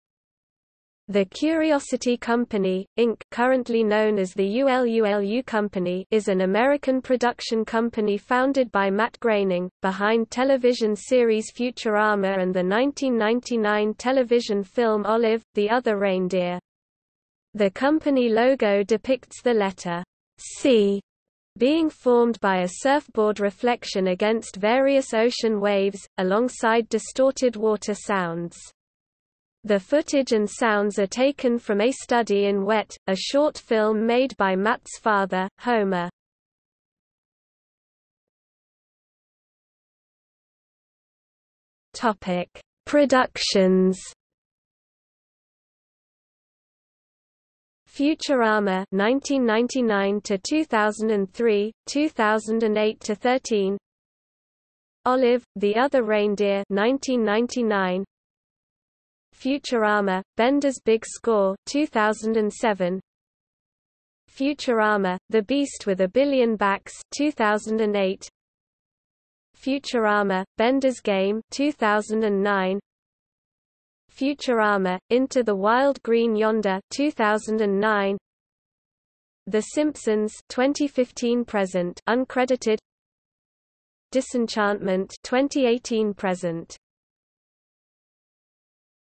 The Curiosity Company | Wikipedia audio article
00:00:38 Productions

- Socrates

SUMMARY
The Curiosity Company, Inc. (currently known as The ULULU Company) is an American production company founded by Matt Groening, behind television series Futurama and the 1999 television film Olive, the Other Reindeer. The company logo depicts the letter "C" being formed by a surfboard reflection against various ocean waves, alongside distorted water sounds. The footage and sounds are taken from A Study in Wet, a short film made by Matt's father, Homer.